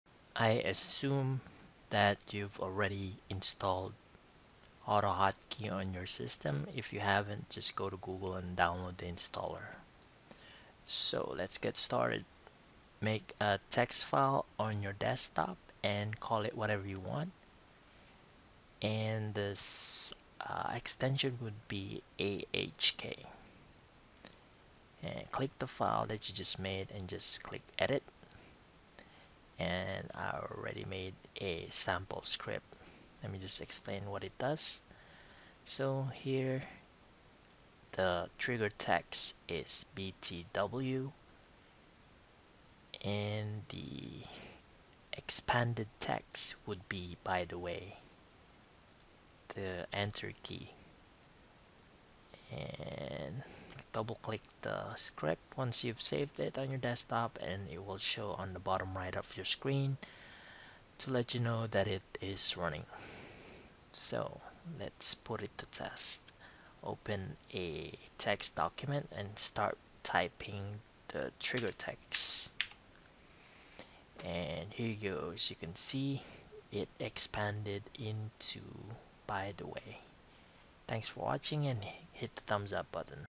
Autohotkey Text Expander Replacement Program Script
Autohotkey script to make a custom text expander or replacement program